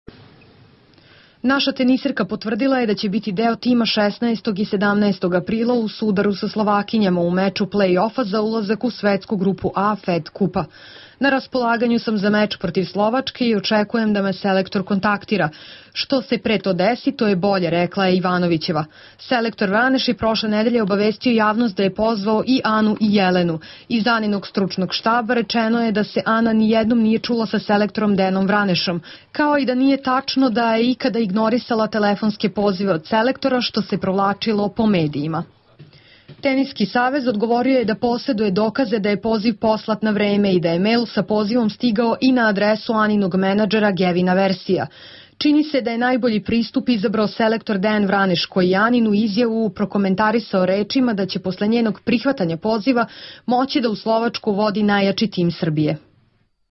Ana: "Igraću za Srbiju"
Srbiju u aprilu očekuje duel sa Slovačkom u FED kupu, a svoje učešće
potvrdila je Ana Ivanović. Ona i Jelena Janković nisu igrale meč sa
Kanadom, kada su briljirale Bojana Jovanovski i Aleksandra Krunić.